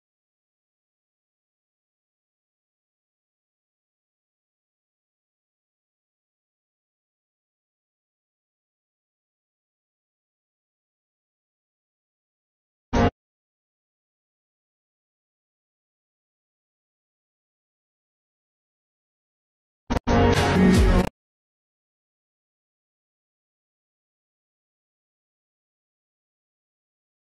🆃🅸🆃🅻🅴 :How to use template in capcut 💯😍editing
How to use template in capcut 💯😍
My name is Hassnain Abbas, In this video, I will show you how to use template in capcut 💯 in very easy way follow for more.